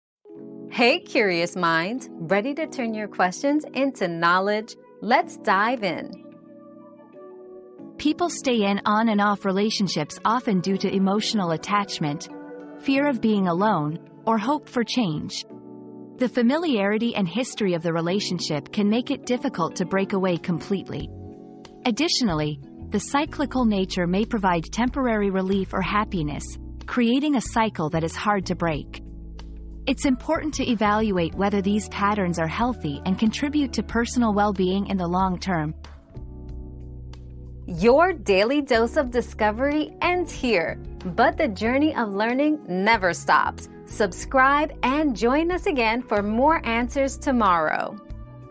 Why do people stay in on-and-off relationships?
Breaking the Cycle: On-and-Off Relationships Explained 👉 On-Off Relationships 👉 Discover why people struggle to break away from on-and-off relationships, from emotional attachment to fear of being alone. Learn how to evaluate if these patterns are truly healthy for your well-being.

Laura S. Harris (2024, May 17.) Why do people stay in on-and-off relationships?
    🌐 AskAbout.video